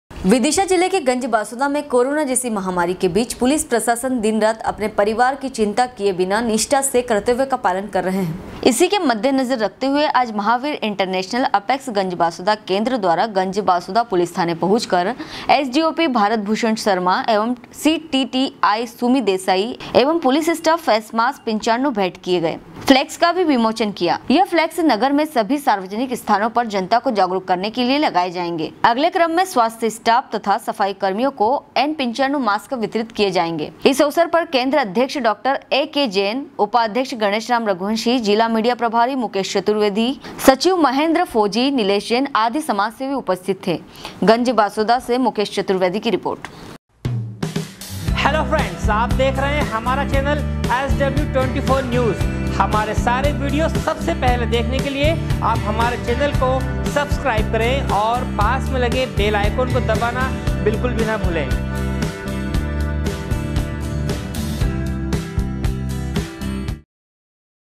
ganjbasoda- पुलिस प्रशासन दिन रात अपने परिवार की चिंता किए बिना निष्ठा से कर्तव्य पालन कर रहा है ।
विदिषा जिले के गंज बासौदा में कोरोना जैसी महामारी के बीच पुलिस प्रशासन दिन रात अपने परिवार की चिंता किए बिना निष्ठा से कर्तव्य पालन कर रहा है । गंज बासौदा से मुकेश चतुर्वेदी की रिपोर्ट